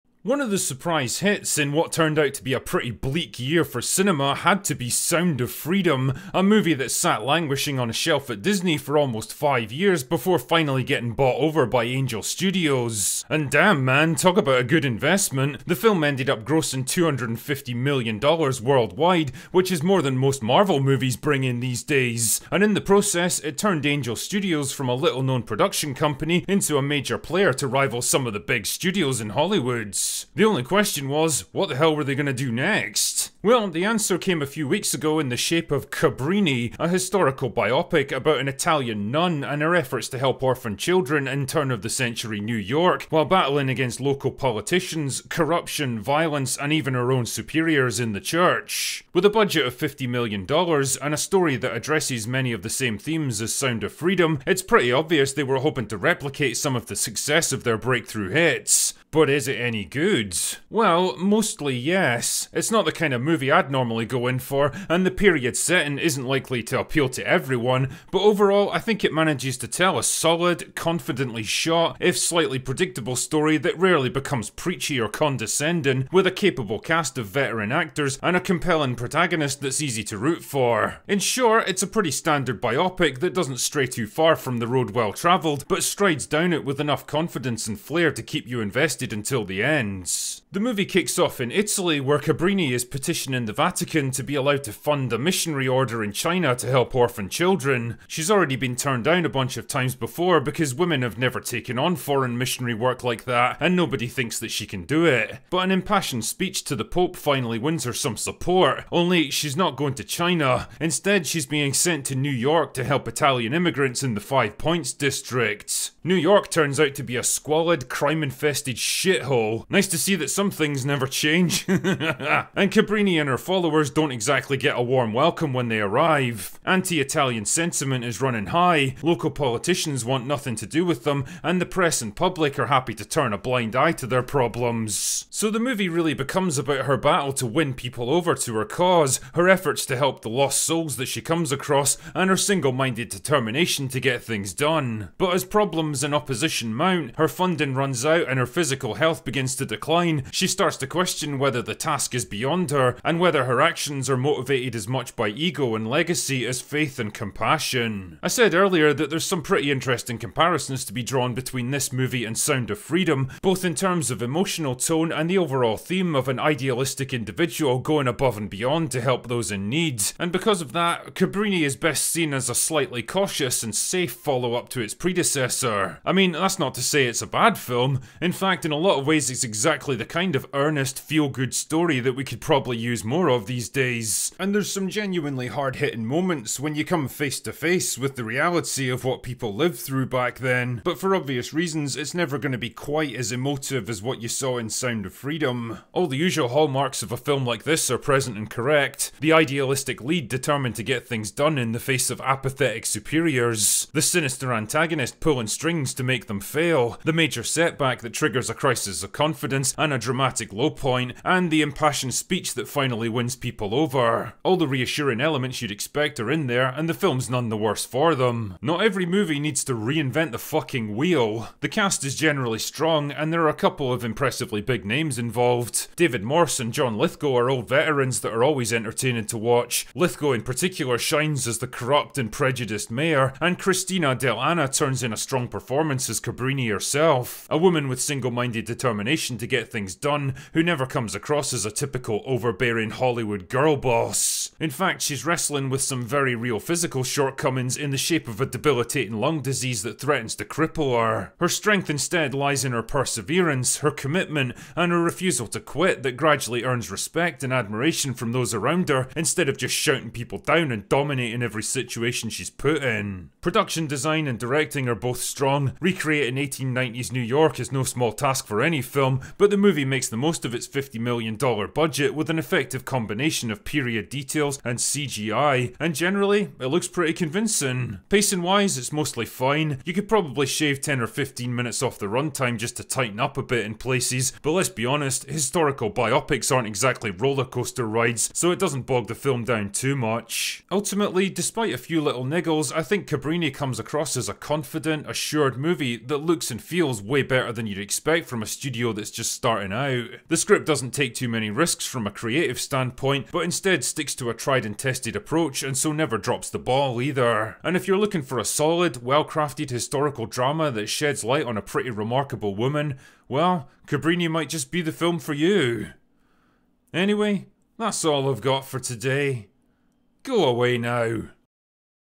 Drinker's Extra Shots - Cabrini
Since the success of Sound of Freedom, I've been curious to see what Angel Studios would do next. And that turned out to be Cabrini, a real-life historical drama about a woman sent to 1890's New York to help the helpless.